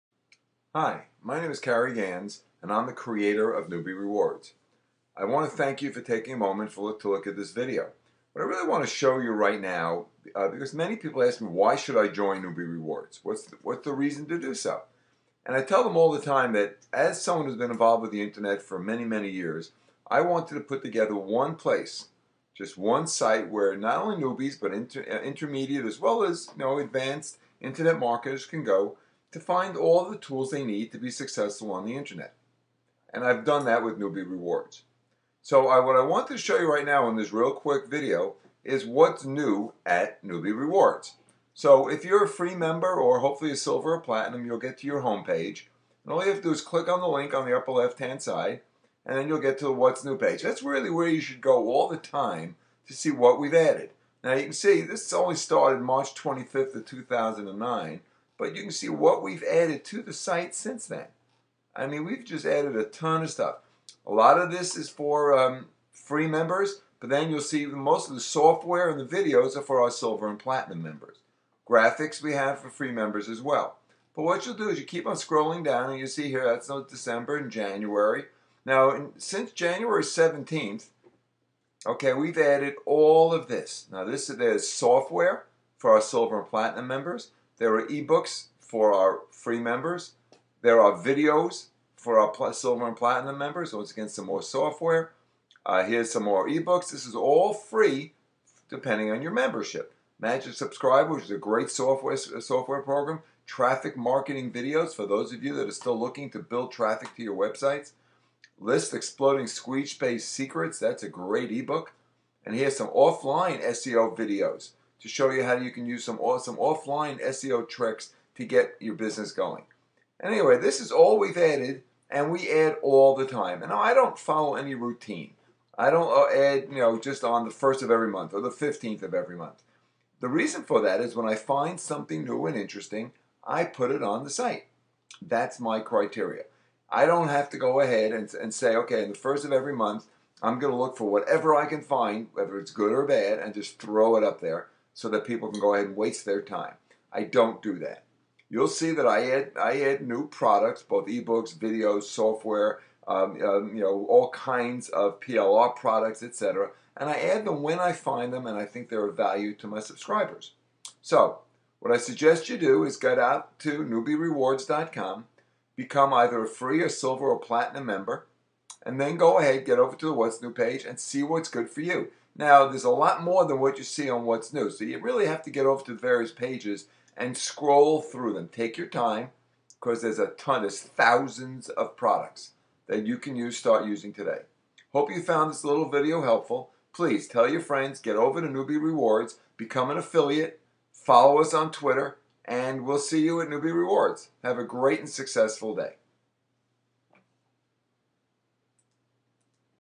What's New at Newbie Rewards
If you're an Internet Marketer, have an Online Business or are thinking of Starting an Online Business. Newbie Rewards has what you need to be successful. Watch this brief video to see what is new at Newbie Rewards.